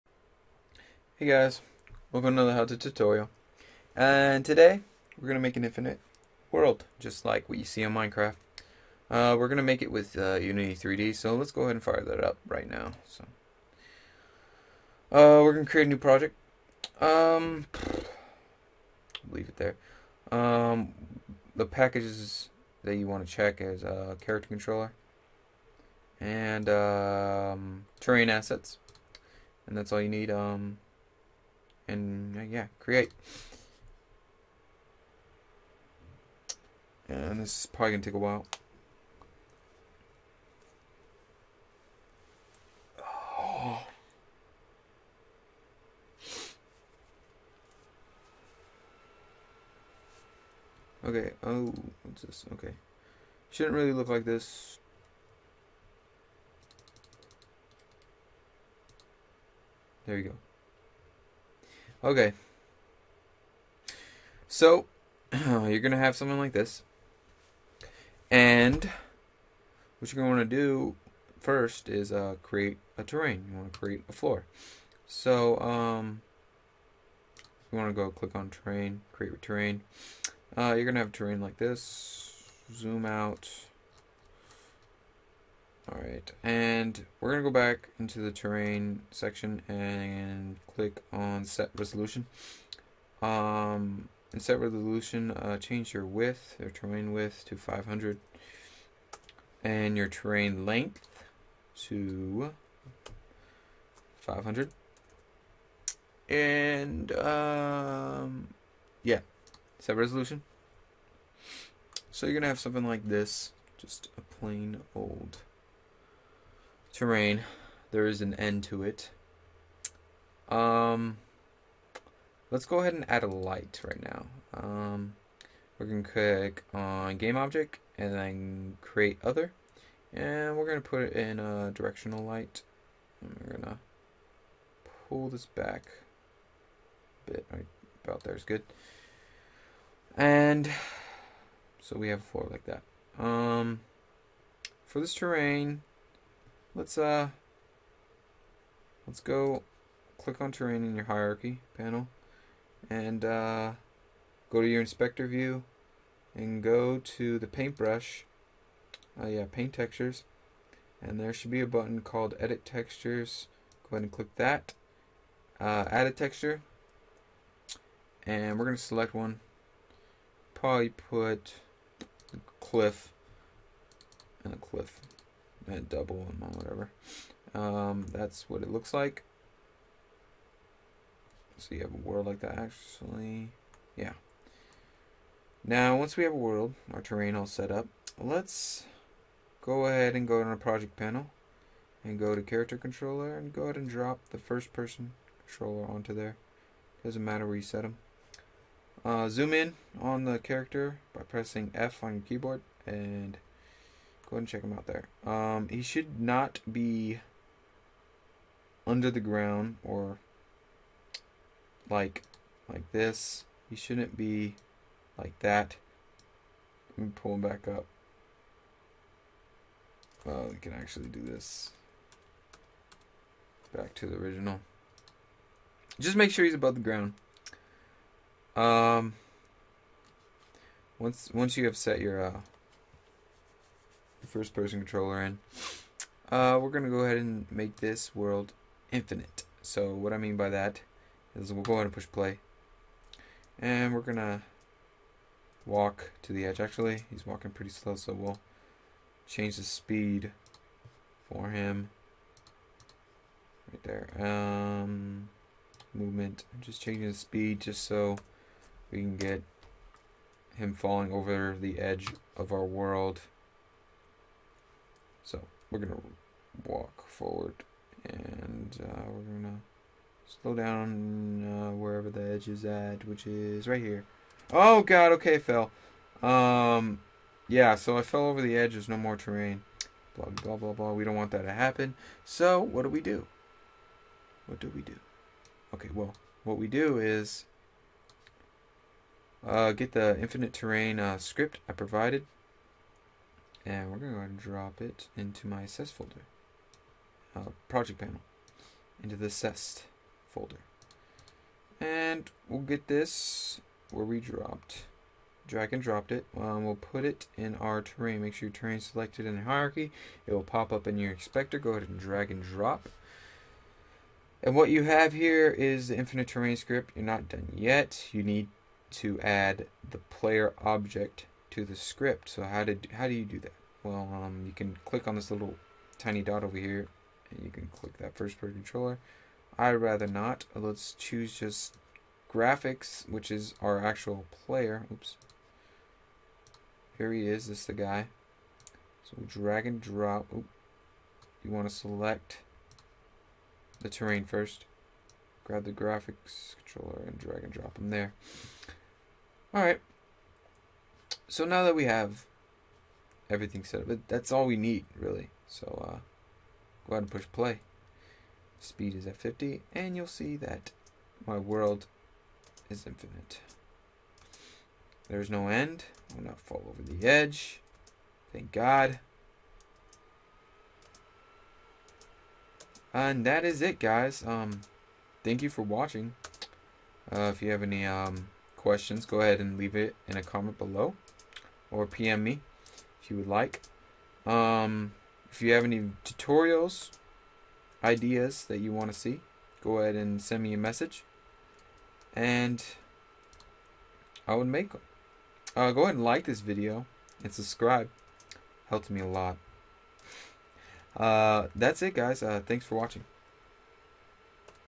How to make an Infinite world in Unity3d
I will show you how to make an infinite world like minecraft.  Show you how to add lighting. I will also show you how to add texture to your terrain.